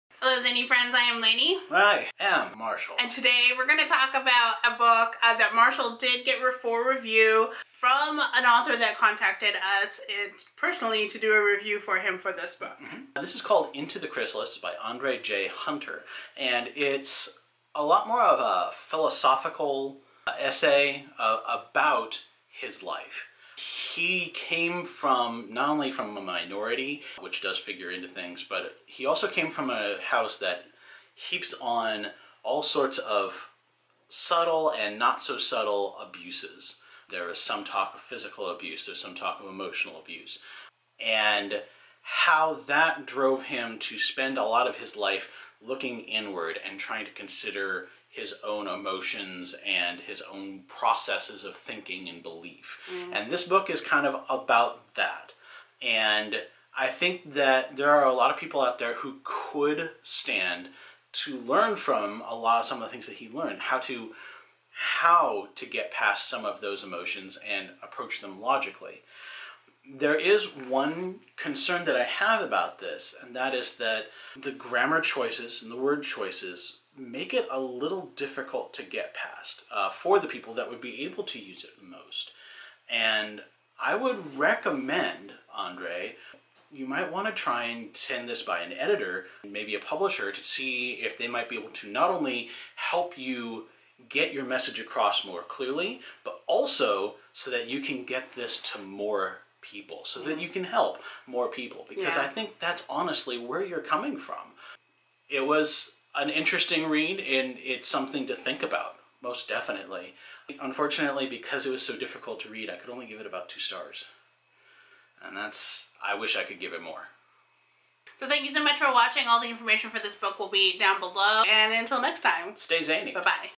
Book Review: Into the Chrysalis | Andre J. Hunter
📖 BOOK REVIEW⠀📚
BOOK: Into The Chrysalis
AUTHOR: Andre J. Hunter
Stars: ⭐⭐
Published: April 20, 2020